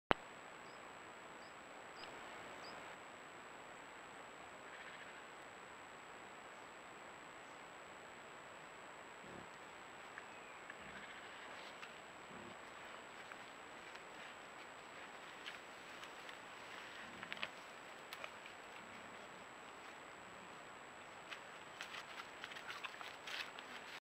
Fototrampeo de zorro y jabalí (Sus scrofa y Vulpes vulpes)
Corto pero bonito vídeo en el que vemos un ejemplar de zorro y un joven jabalí.